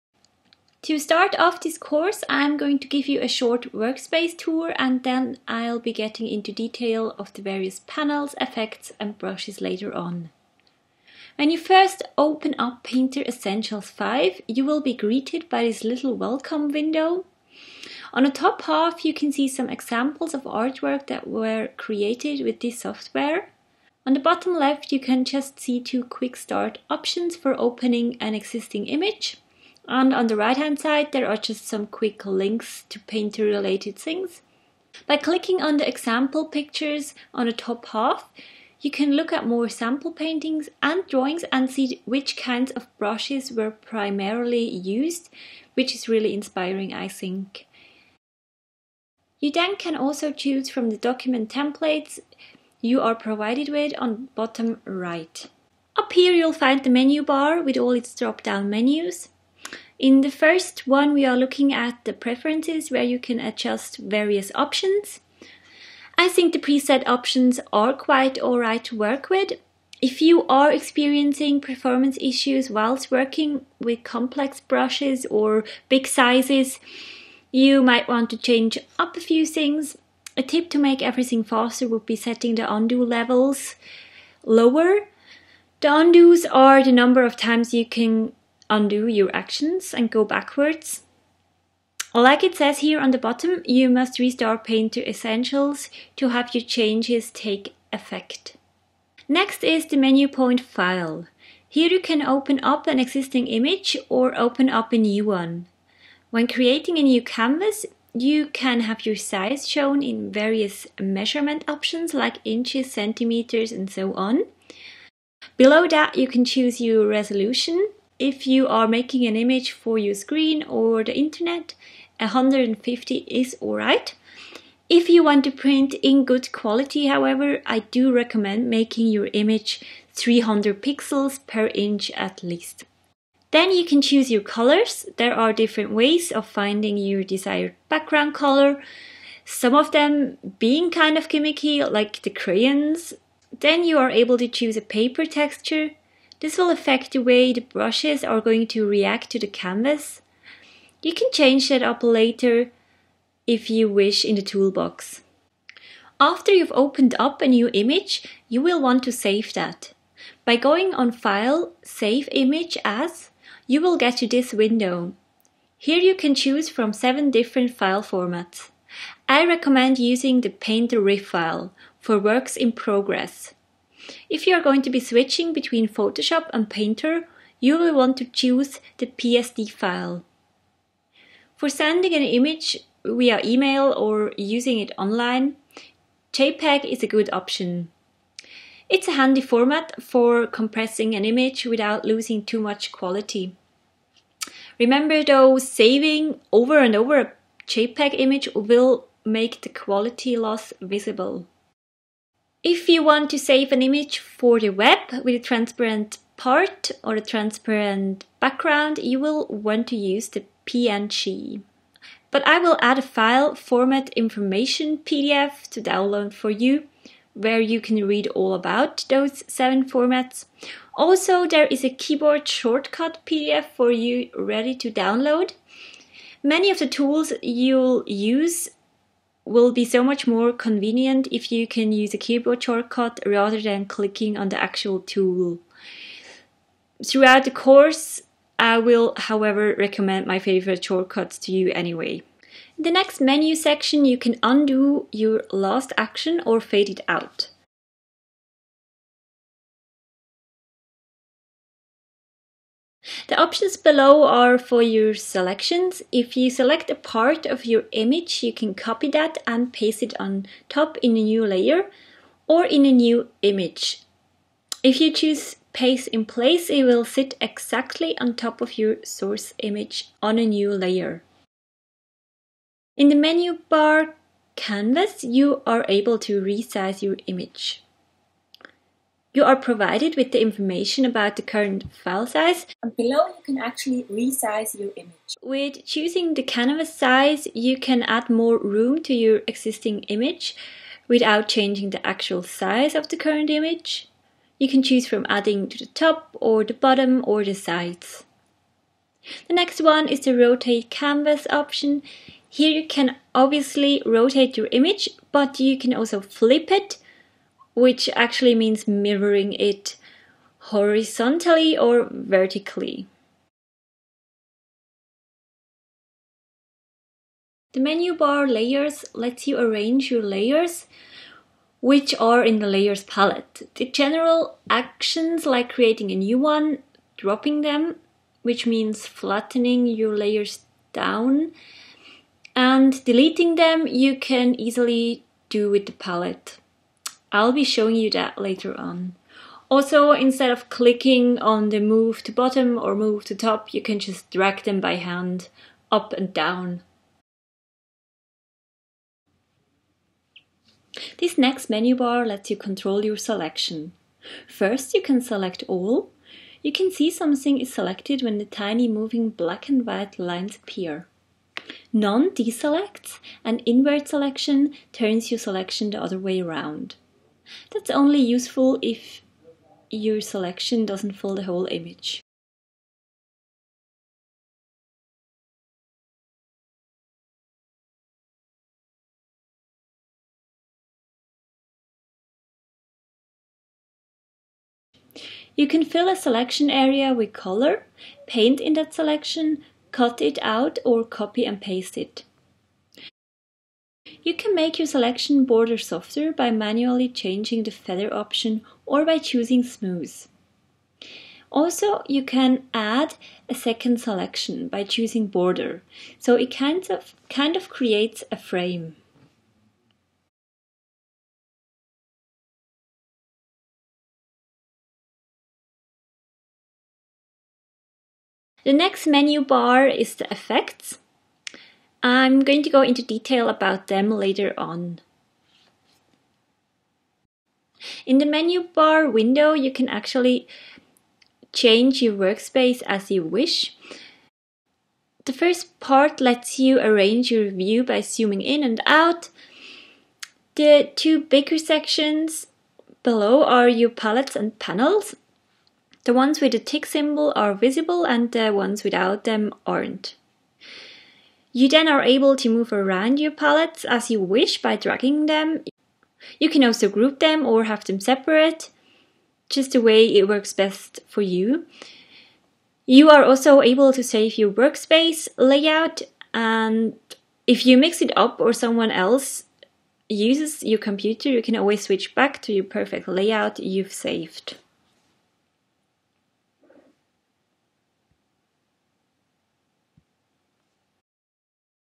Learn to paint with Painter Essentials 5 : Workspace Overview
Learn to paint with Painter Essentials 5
Digital Painting with Painter Essentials 5 in an easy step-by-step course.